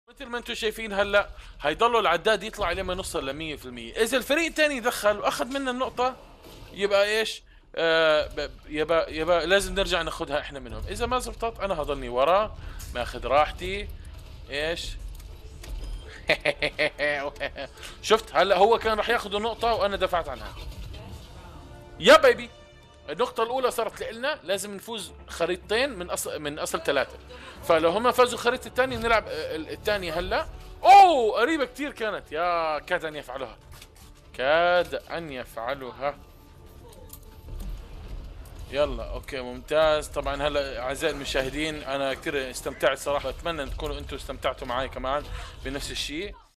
جاري اللعب | من منكم يعرف اسم هذه اللعبة؟
في البرنامج الأول الذي حيث يستعرض محترفوا ألعاب الفيديو مواهبهم - يمكنكم مشاهدته مباشرة عبر البث الحي على يويتوب الساعة 00:30 KSA يومياً في رمضان عبر 👈